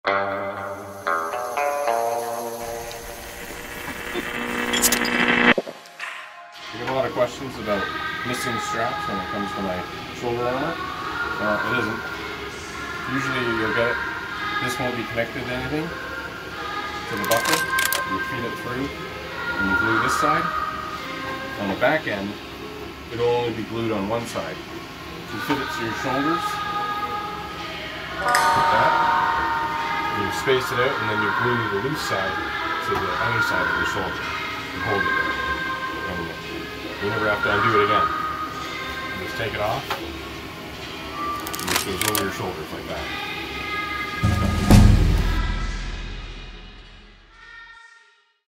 Shoulder armor assembly
Brief explanation on how to use my shoulder armor supplied from my Etsy store!